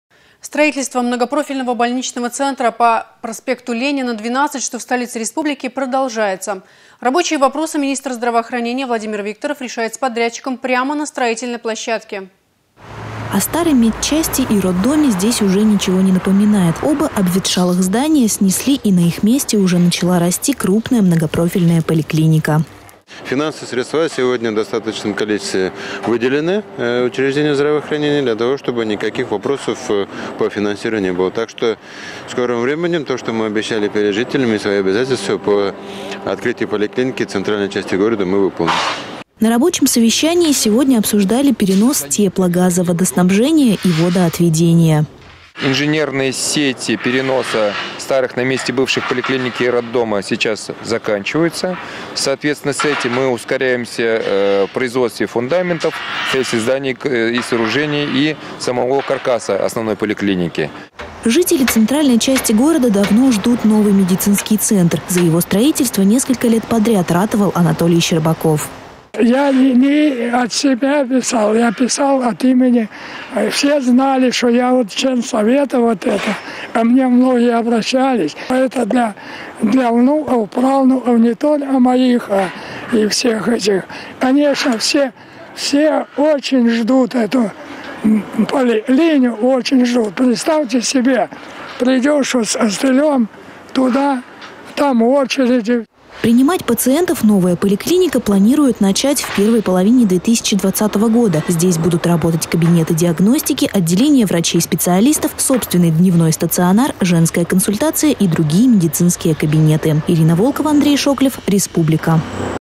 Строительство многопрофильного больничного центра в столице республики продолжается.
Строительство многопрофильного больничного центра по Ленина, 12, что в столице республики,  продолжается. Рабочие вопросы министр здравоохранения Владимир Викторов решает с подрядчиком  прямо на строительной площадке.